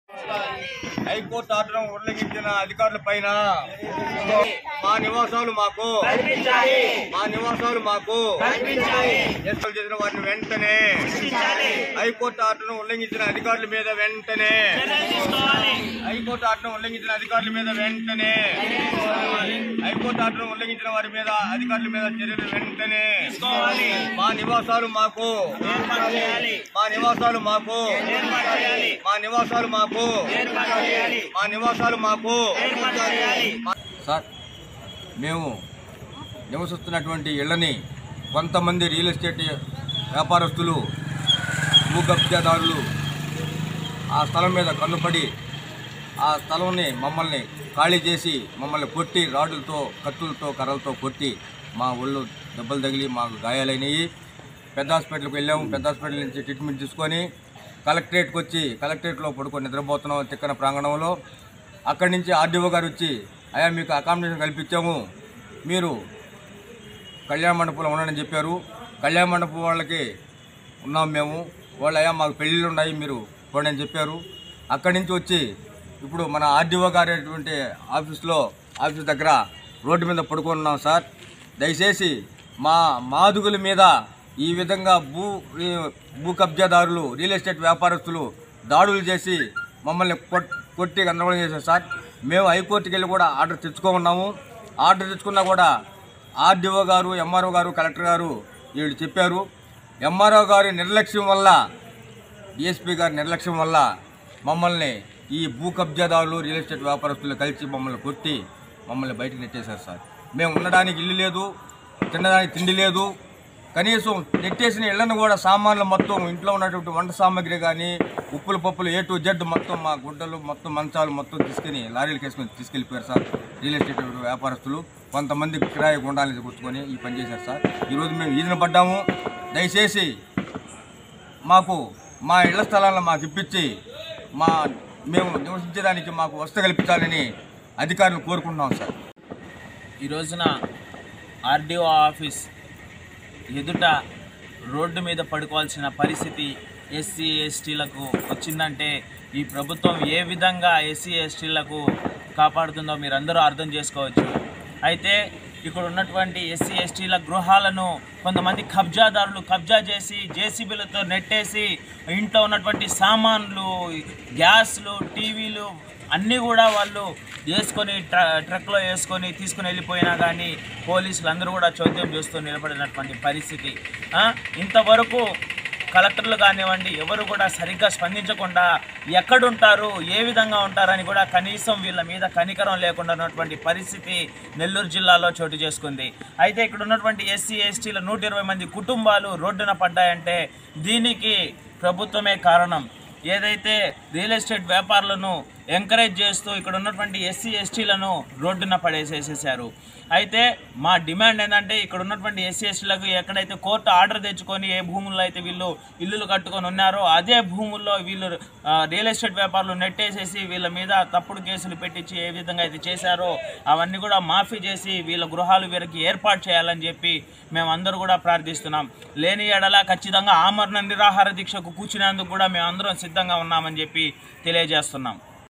అర్ధరాత్రి ఆర్టిఓ ఆఫీసు ఎదుట నక్క గోపాల్ నగర్ వాసులు - Mynews
నెల్లూరు రూరల్ నక్క గోపాల్ నగర్ లో గుడిసెలు వద్ద జరిగిన ఘర్షణ అందరికీ తెలిసిందే ఈ నేపథ్యంలో గుడిసెలు కోల్పోయిన వారంతా నిలువ నీడలేని కి మమ్మల్ని ఆదుకోండి అంటూ ఆర్డిఓ ఆఫీస్ ఎదుట అర్ధరాత్రి అయినా న్యాయం చేయాలంటూ ఆవేదన వ్యక్తం చేస్తున్నారుMyNews